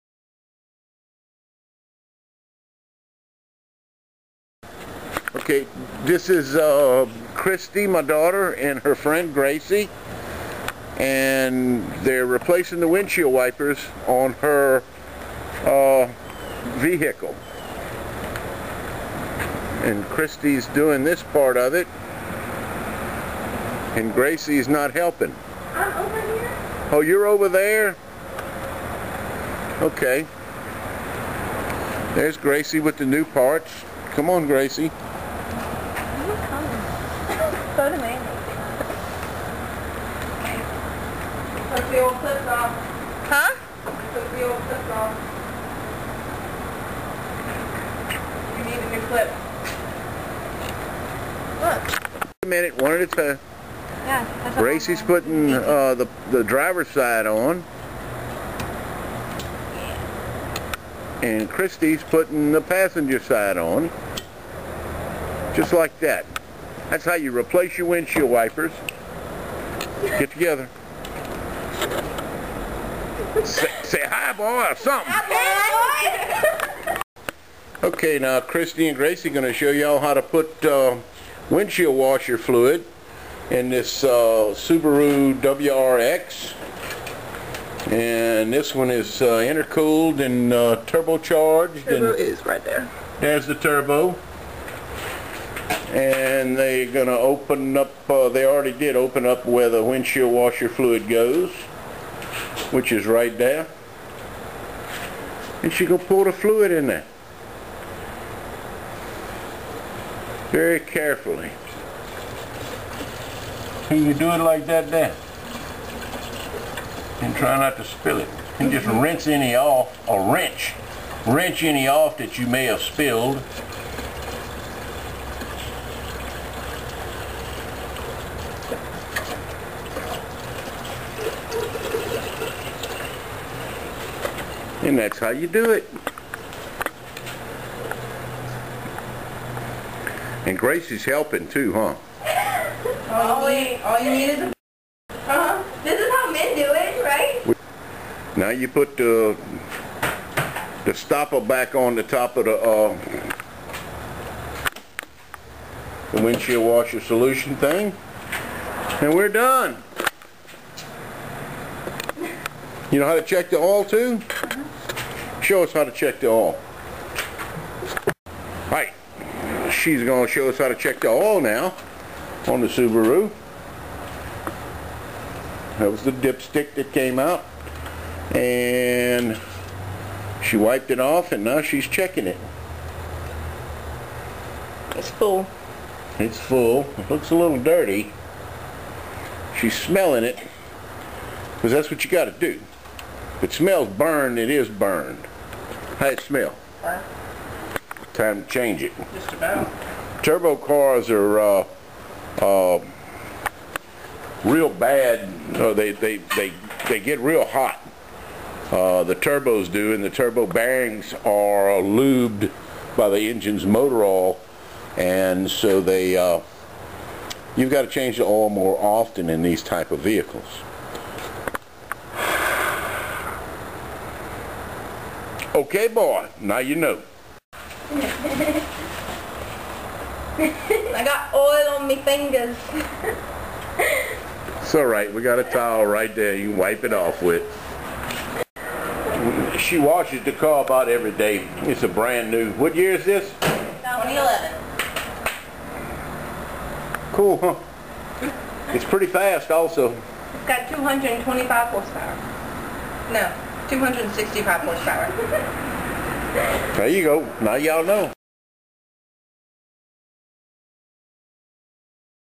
Cajun girls working on their cars
changing the wiper blades, checking the oil & adding windshield washer fluid on a car